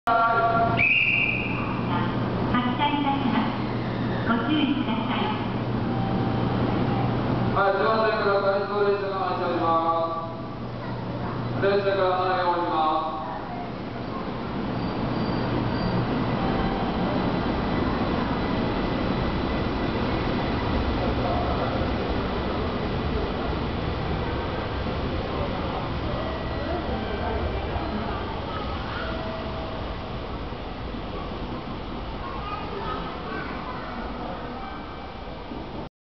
新幹線923系T4編成＠2010年04月13日新富士駅
2010年04月13日 19：13新富士駅発東京行き：T4編成こだま検測